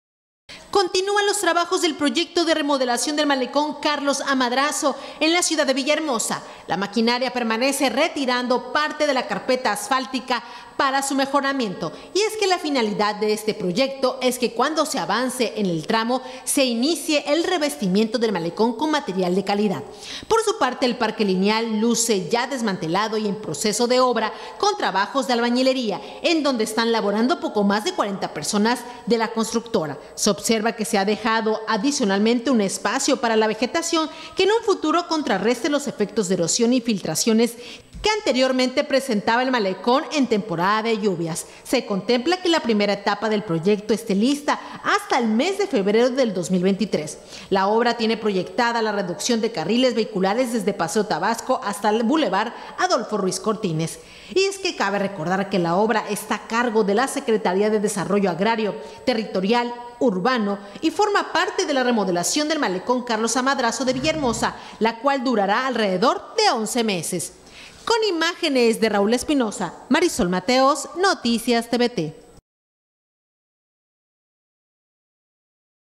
Continúan los trabajos de remodelación del malecón "Carlos A. Madrazo"
NoticiasTVT🟣 Siguen en marcha trabajos del proyecto de remodelación del malecón Carlos A. Madrazo; con maquinaria pesada se retira la carpeta asfáltica que habrá de sustituirse por concreto hidráulico. El Parque Lineal luce desmantelado.